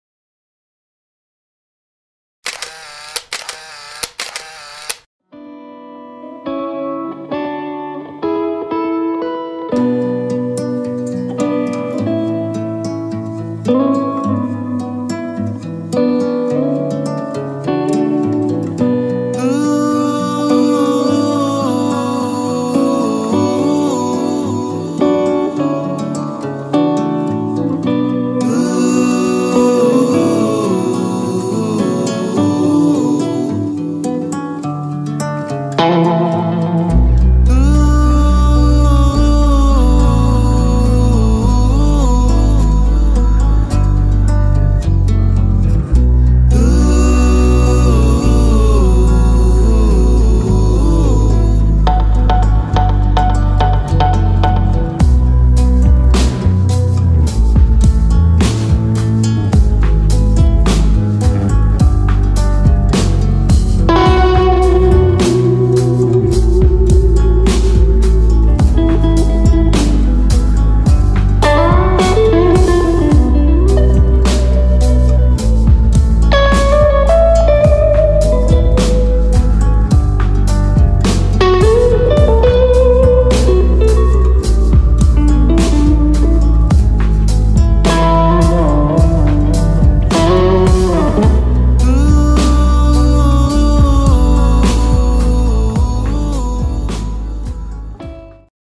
The Ghan transformed into a moving work of art
The Ghan has been transformed into a spectacular piece of moving art, with three carriages of the world-famous train wrapped in a stunning artwork, Crossroads, as it travels from Darwin to the Red Centre. The artwork is a collaboration between Tourism Northern Territory, NT Major Events Company (NTMEC), Journey Beyond and AGB Events with the help of Aboriginal artist Chantelle Mulladad, Keringke Arts.